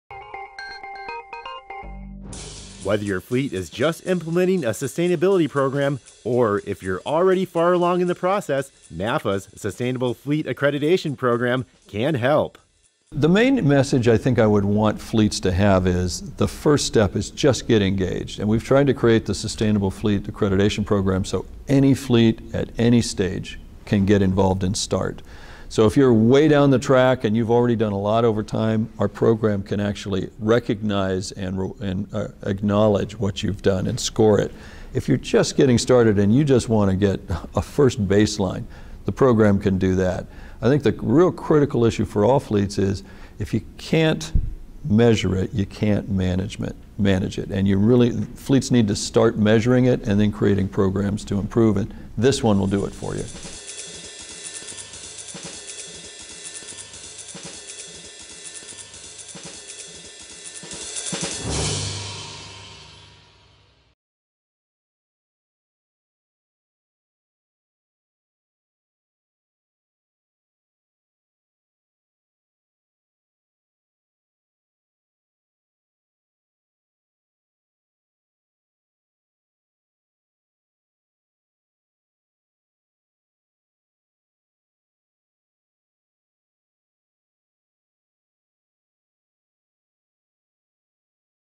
Sustainable Fleet Accreditation - Just Get Involved | BILL VAN AMBURG | Fleet Management Weekly
Whether your fleet is just implementing a sustainability program - or if you’re already far along in the process - NAFA’s Sustainable Fleet Accreditation Program can help.

BILL VAN AMBURG
Senior Vice President
CALSTART

151103FmwBVAmburg01